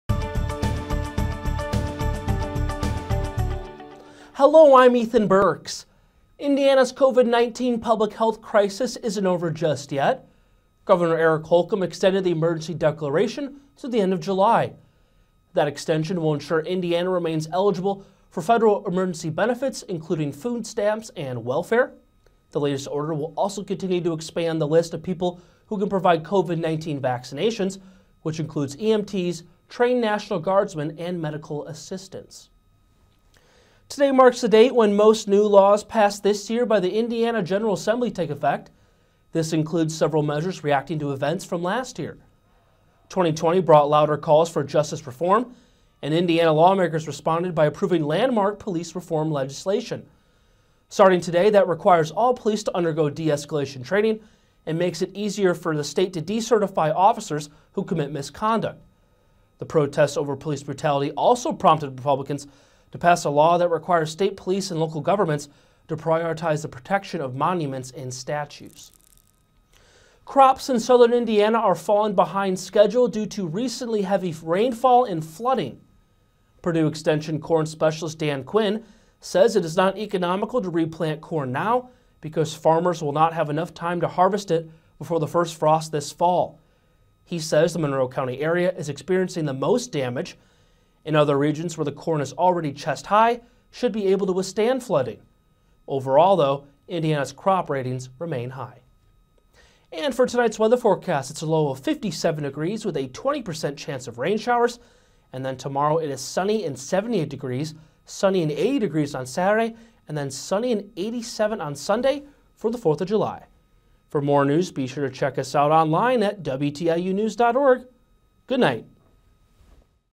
WTIU Newsbreak, July 1, 2021
WTIU's Ethan Burks reports on Indiana extending its public health emergency declaration, new Indiana laws coming into effect and crops in Southern Indiana falling behind schedule.